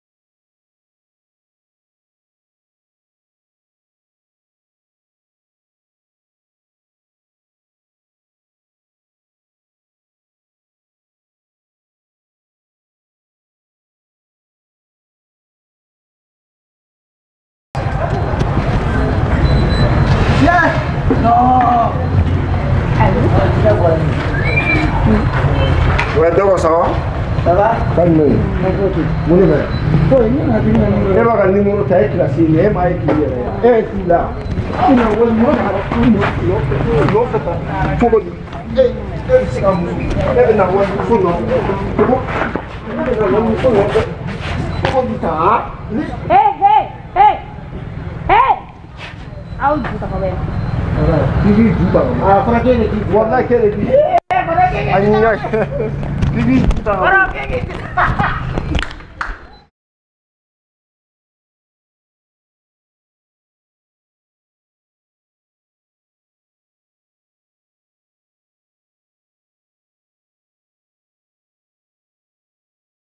roi 12 12 dans bagare de la rue par BEN BD PROD
L'artiste Comédien Alassane Sam dit ROI 12 12 est un jeune Comédien de Bamako qui joué dans le groupe YELEBOUGOU et qui est très très aimé de nos jours partout au Mali et le voilà dans bagare de la ruel pour vous les fans avec le soutien de son LABEL UNIVERSEL BEN BD PRODUCTION INTERNATIONALE aux contacts: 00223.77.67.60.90 ou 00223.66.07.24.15 ou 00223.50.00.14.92 par E-mail